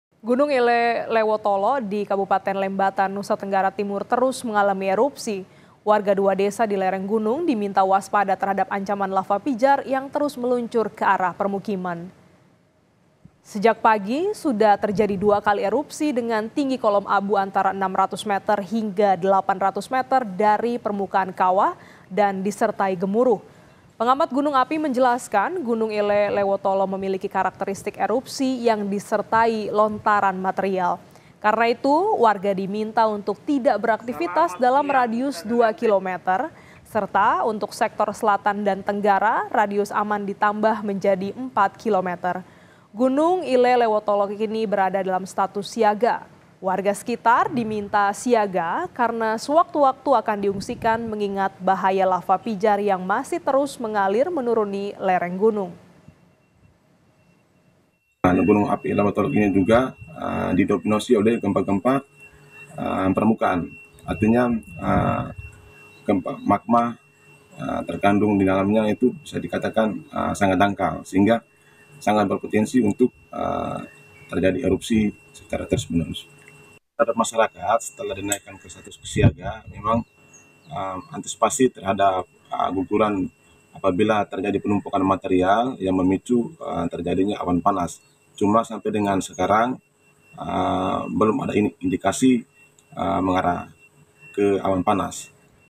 Terus Keluarkan Kolom Abu dan Lava Pijar, Warga Sekitar Gunung Ile Lewotolok NTT Diminta Siaga!
LEMBATA, KOMPAS.TV - Gunung Ile Lewotolok di Kabupaten Lembata, Nusa Tenggara Timur (NTT), terus mengalami erupsi.

Warga dua desa di lereng gunung diminta waspada terhadap ancamam lava pijar yang terus meluncur ke arah permukiman.

Sejak pagi, sudah terjadi dua kali erupsi dengan tinggi kolom abu antara 600-800 meter dari permukaan kawah, yang disertai gemuruh.

Pengamat Gunung Api menjelaskan, Gunung Ile Lewotolok memiliki karakteristik erupsi yang disertai lontaran material.

Karena itu, warga diminta untuk tidak beraktivitas dalam radius 2 kilometer.

Sementara untuk sektor selatan dan tenggara, radius aman ditambah menjadi 4 kilometer.

Gunung Ile Lewotolok kini berada di status siaga.

Warga sekitar diminta siaga, karena sewaktu-waktu akan diungsikan mengingat bahaya lava pijar yang masih terus mengalir menuruni lereng gunung.